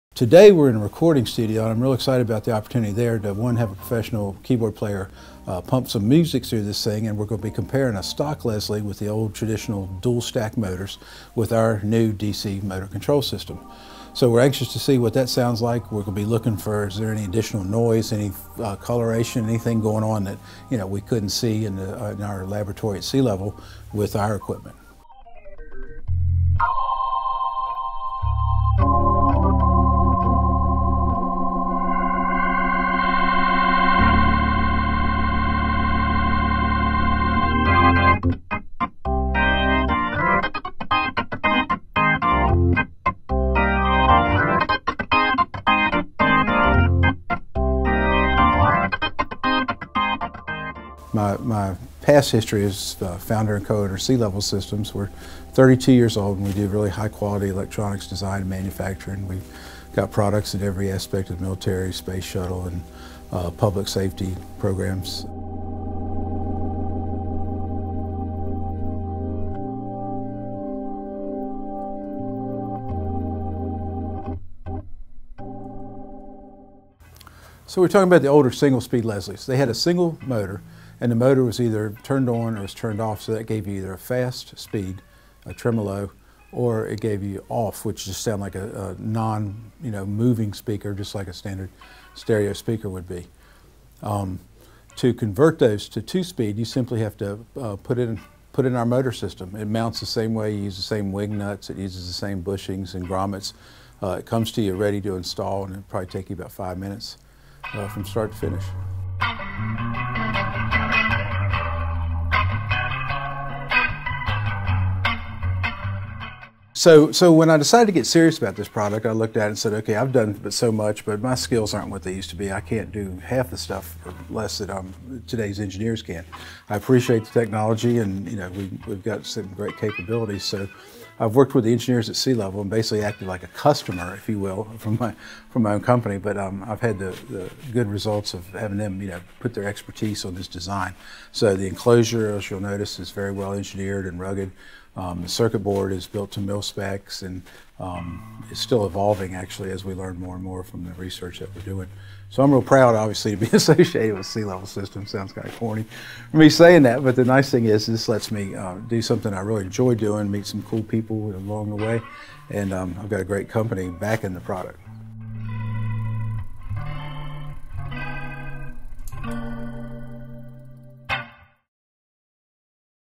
BookerLAB Revolution: The Advanced Motor Controller
Our desire for pristine, vintage sound quality led us to develop Revolution: The Advanced Motor Controller. Using speed measurement software, we analyzed many Leslie speaker models, specifically the 122, the 21H and the 147. We captured motor response time from stop to slow to fast and back. We saved and plotted RPM vs. time for each rotor, the motor RPM and the slope of the ramp. Revolution emulates those conditions precisely.